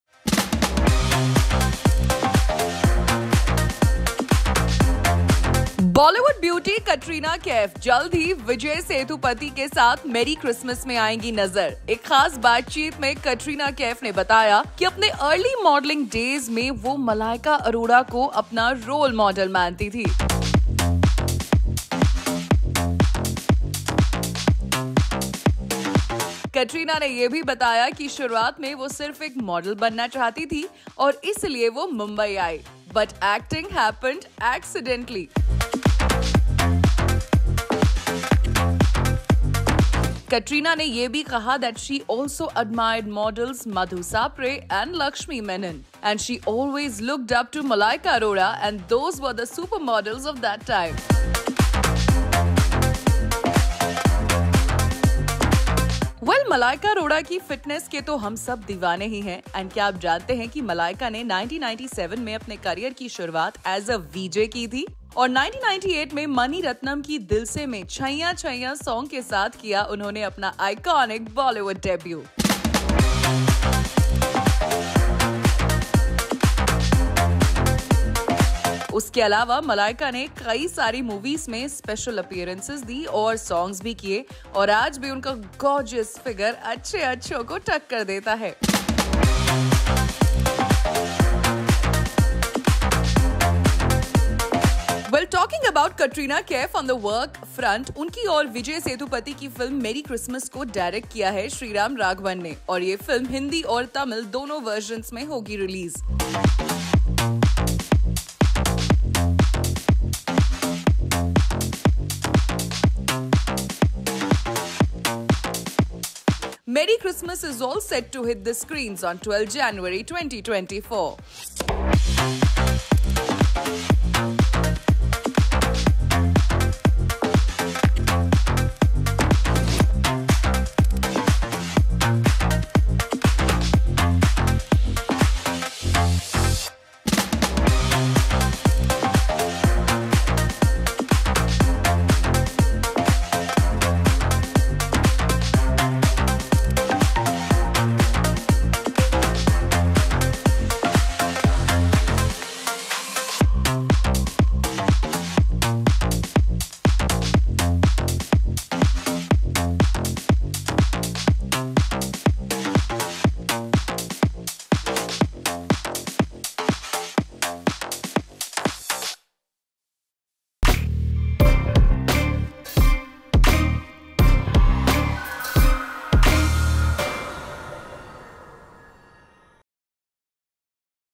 Katrina Kaif's BIG statement on Malaika Arora as she recalls her early modelling days
Katrina Kaif recently revealed some interesting details about her early modelling days and revealed she had no plans of becoming an actor. Here's what Katrina said about Malaika Arora during her recent interview!